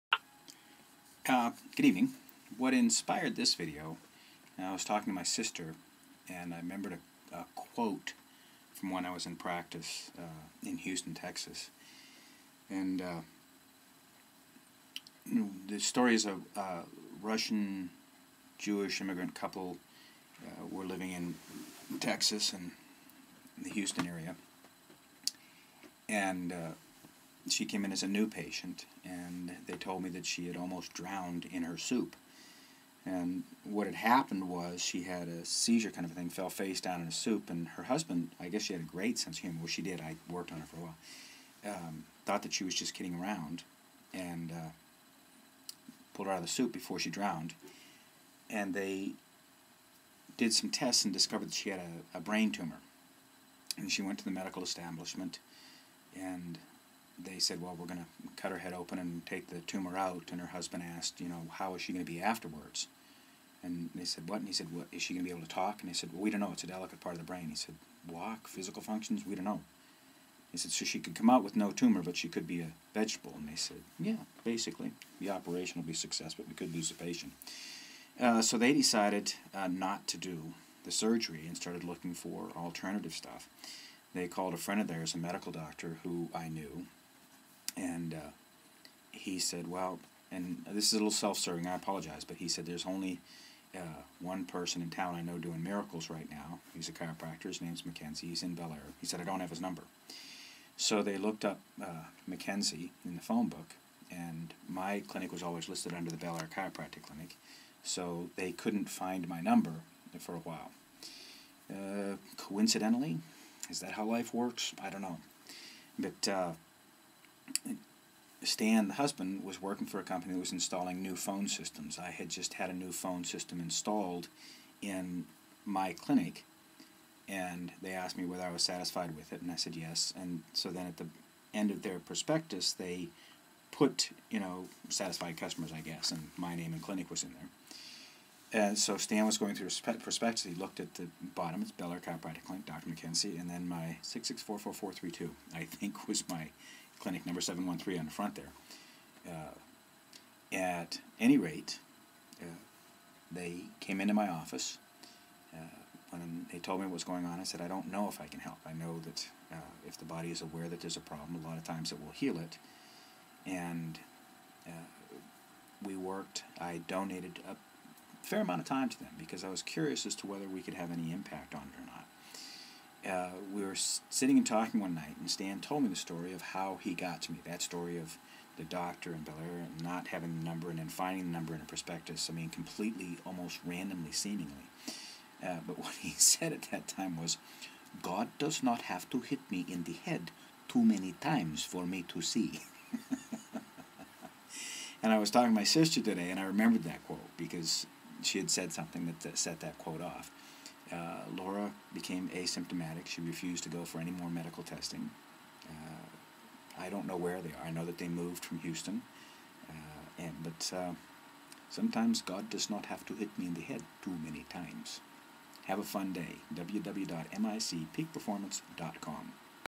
Laura Pshonik • Alternatives to Surgery • Mick MacKenzie
"God does not have to hit me in the head too many times for me to see..."

Browse Mick's book -- Noticing Fixes more than Fixing -- a Handbook for Alternative Practitioners,